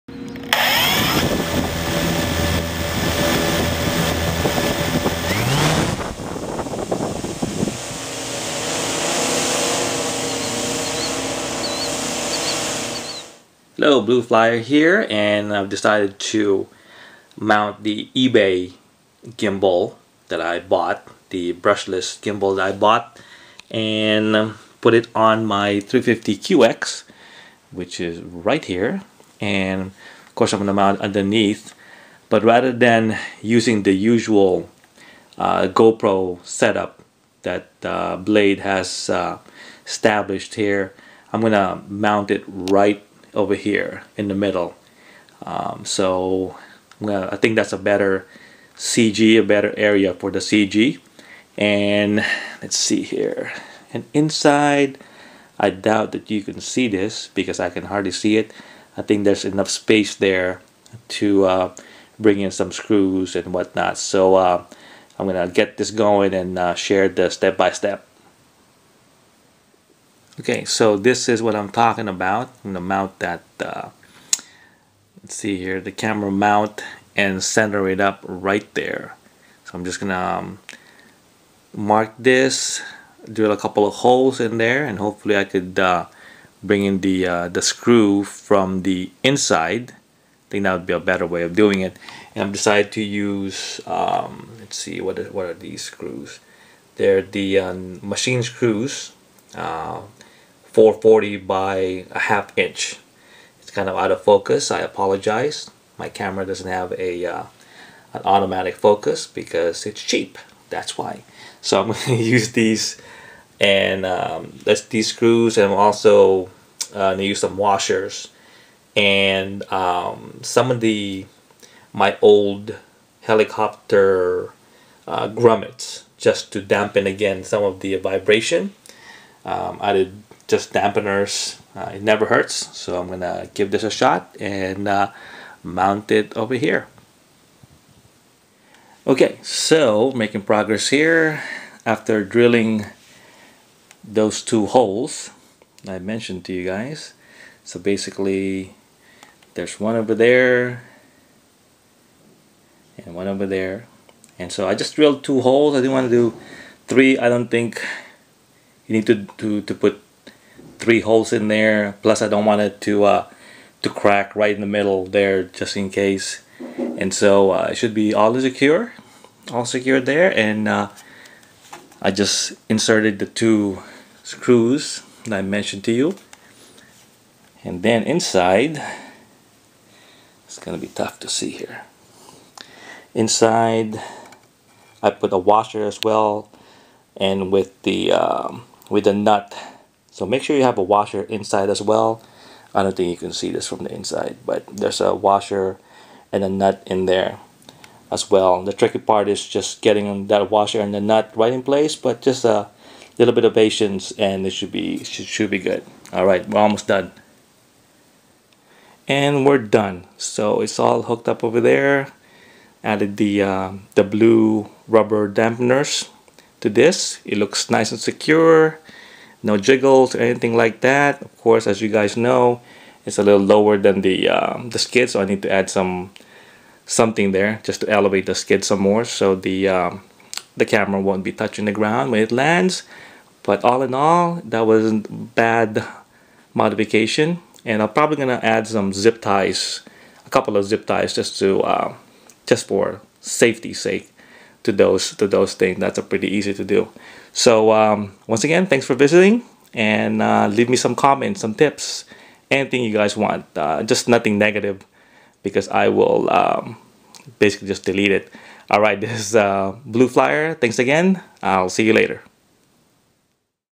Mounting eBay Brushless Gimbal on 350 QX: How-to
Thanks for the PM's, requests and comments about this.  I'm sharing how I mounted the eBay gimbal on to my 350 QX.  After this, I'll be testing it and of course, posting the videos.